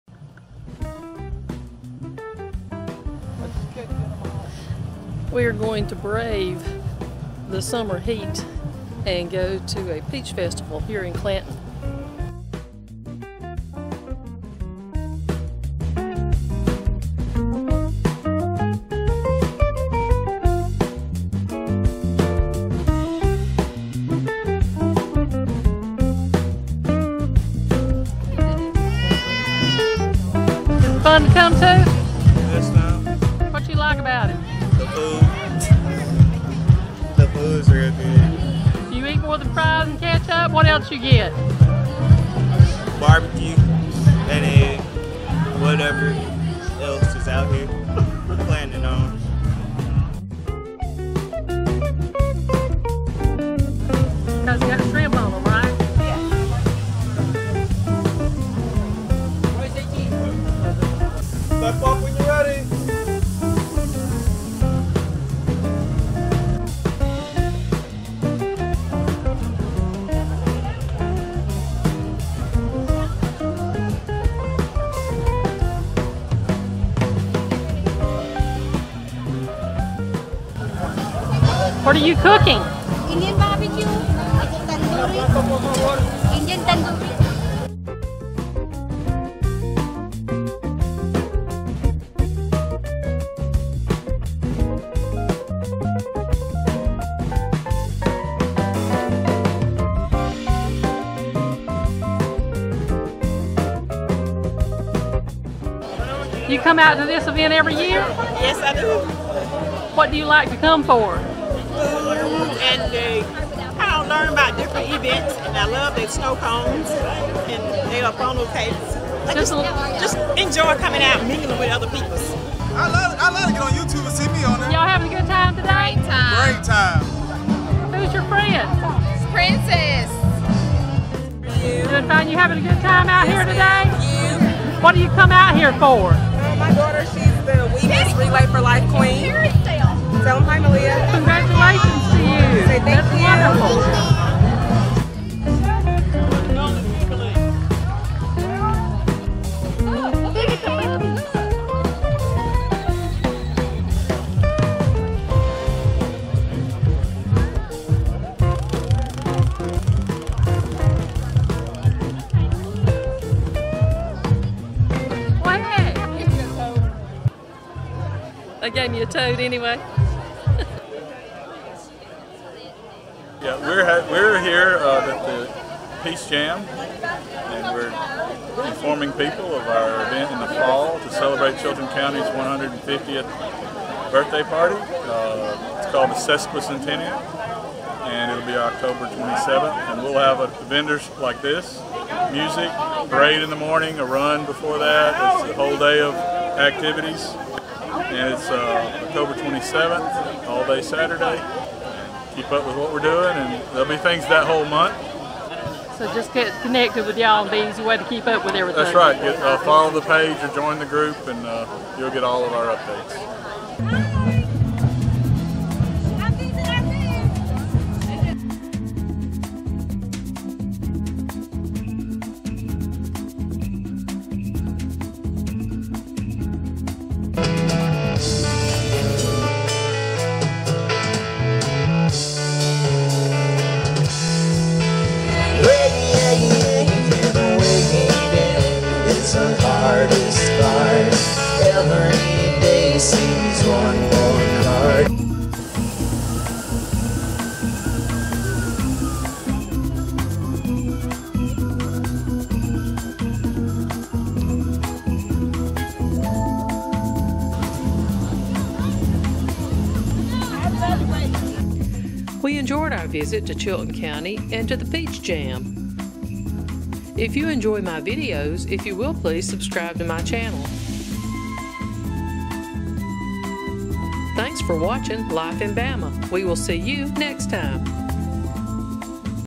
Peach Jam Jubilee in Chilton County|Life In Bama
Hello everyone, please note I have incorrectly identified the name of the band playing at the event. Their name is Cotton Bird. There is an in video link that will take you to their You Tube channel if you would like to watch some of their videos!
Having a Peachy Time at the Peach Jam Jubilee at Clanton Park. It is a hot day in Central Alabama but we brave the heat to see what is going on at the Peach Jam Jubilee! Chilton County is a major producer of peaches and eating their peaches is a summertime tradition in Alabama. There is food galore at this event! We talk with some of the visitors to see why they come to this event and see some of the sights.
Hope you enjoy seeing what is happening at the Peach Jam.